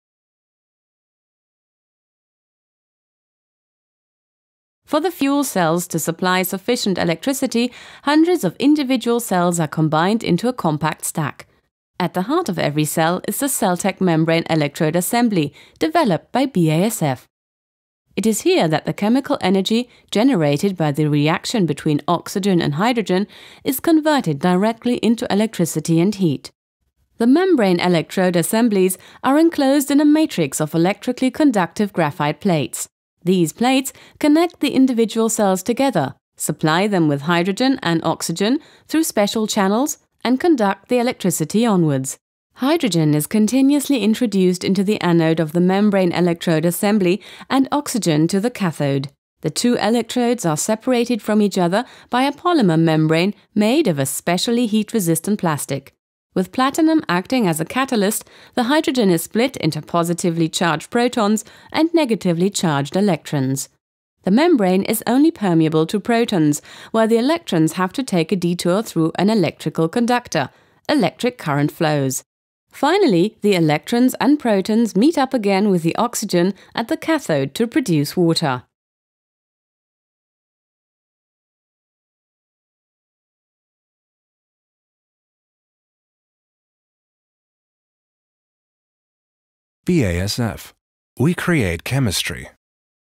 How does a fuel cell work?
Hydrogen and oxygen in fuel cells react in a controlled manner to produce water and in doing so, generate electricity and heat very efficiently. High-temperature PEM fuel cells are particularly suitable for operation with reformate gas from methanol, bio-gas or natural gas, for example. Fuel cells in aircraft, cars, trains and ships can generate electricity rapidly and with low exhaust emissions.